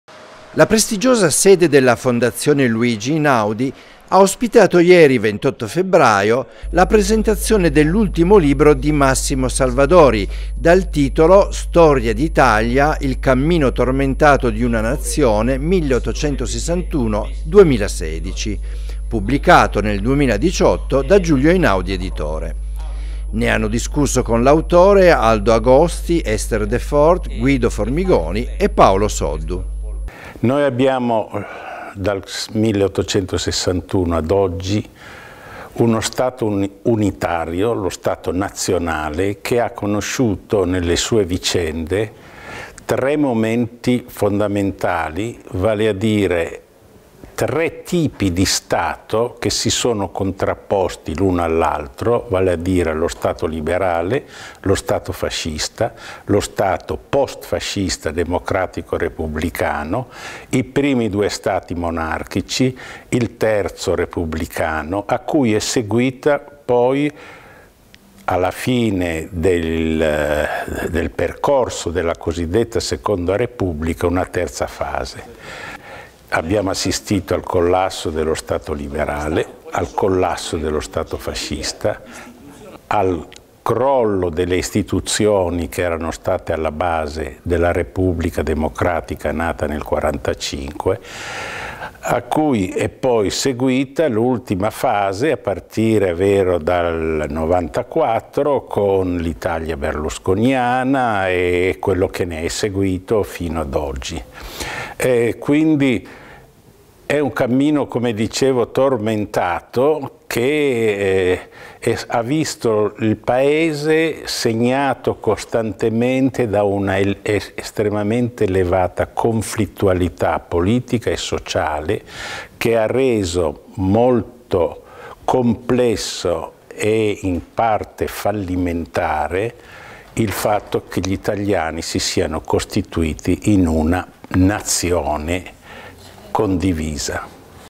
La "Storia d'Italia" di Massimo Salvadori
Presentato nella prestigiosa sede della Fondazione Einaudi il nuovo libro edito nel 2018 “Storia d’Italia. Il cammino tormentato di una nazione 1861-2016” dello storico piemontese Massimo L. Salvadori.